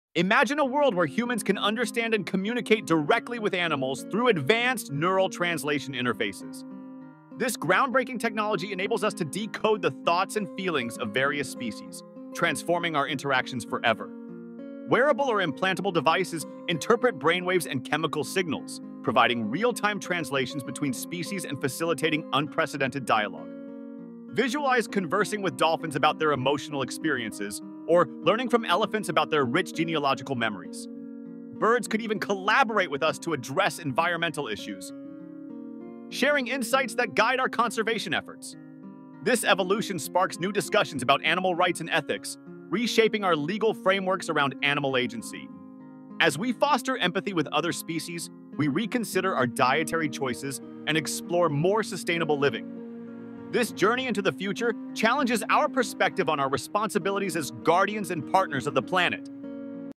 Talking to Animals May Soon Be Real
What if you could talk to animals? Discover the tech thats making it possible.